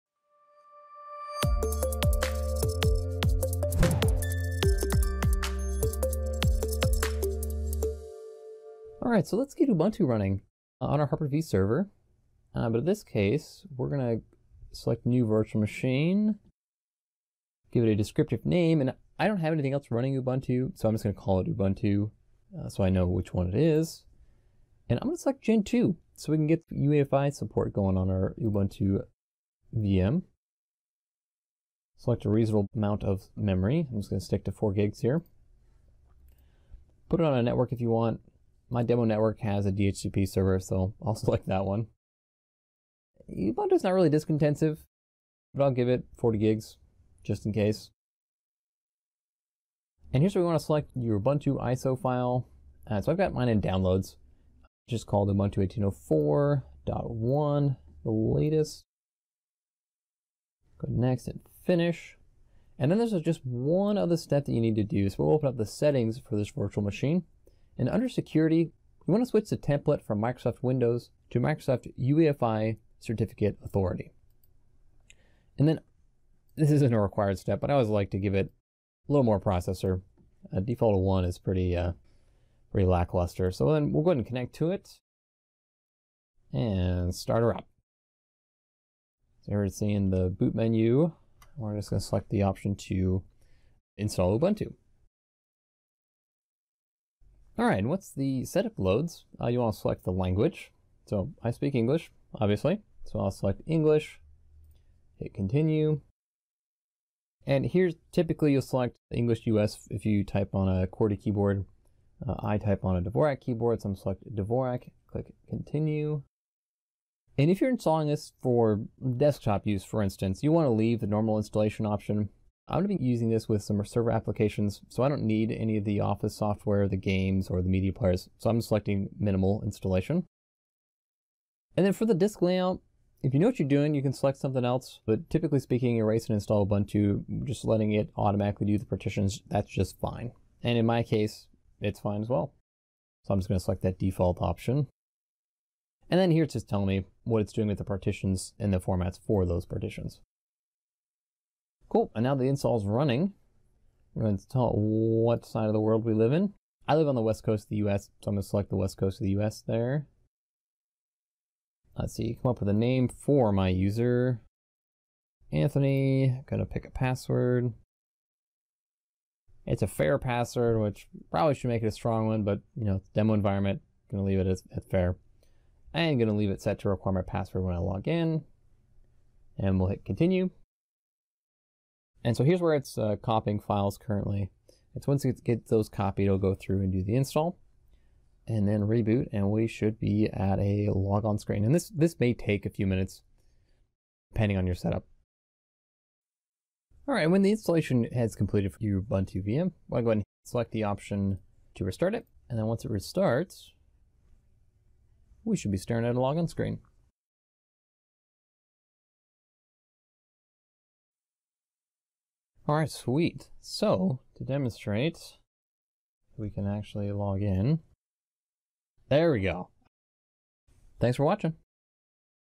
How To Install Ubuntu In A Hyper-V Generation 2 Virtual Machine
Although it's now possible to run several types of Linux distributions on Windows Hyper-v, you were generally only able to use Generation-1 guests machines due to lack of support of  Microsoft UEFI secure boot template.

Hyper-v Generation 2 virtual machine offers many more features and options available to the guest OS and most modern Linux distributions now support UEFI.

After creating the virtual machine, switching to the Microsoft UEFI Certificate Authority Secure Boot Template will allow Ubuntu and other Linux distributions to boot from the ISO.

Prerequisites include:
Hyper-v Installed on Windows 10 or Server2012 R2 or higher
Ubuntu ISO Image